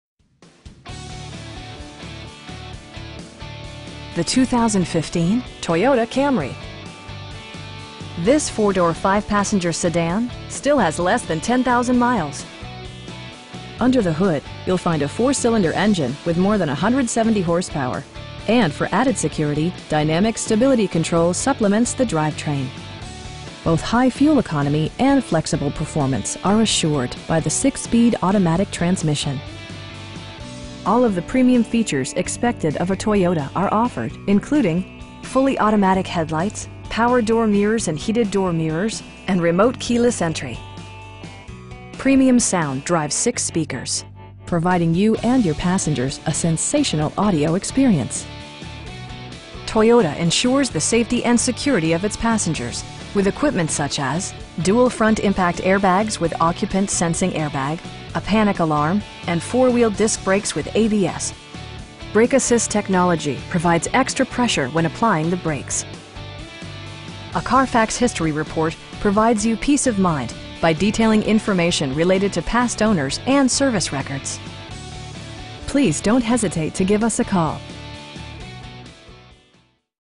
2015 Toyota Camry in Montgomery, AL 36117
Brewbaker Preowned
300 Eastern Blvd.

The 2015 Toyota Camry. This 4 door, 5 passenger sedan still has less than 10,000 miles! Under the hood you'll find a 4 cylinder engine with more than 170 horsepower, and for added security, dynamic Stability Control supplements the drivetrain. Both high fuel economy and flexible performance are assured by the 6 speed automatic transmission. All of the premium features expected of a Toyota are offered, including: variably intermittent wipers, a trip computer, fully automatic headlights, telescoping steering wheel, power door mirrors and heated door mirrors, cruise control, and remote keyless entry. Premium sound drives 6 speakers, providing you and your passengers a sensational audio experience. Toyota ensures the safety and security of its passengers with equipment such as: dual front impact airbags with occupant sensing airbag, head curtain airbags, traction control, a panic alarm, and 4 wheel disc brakes with ABS. Brake assist technology provides extra pressure when applying the brakes. A Carfax history report provides you peace of mind by detailing information related to past owners and service records. We pride ourselves in consistently exceeding our customer's expectations. Please don't hesitate to give us a call.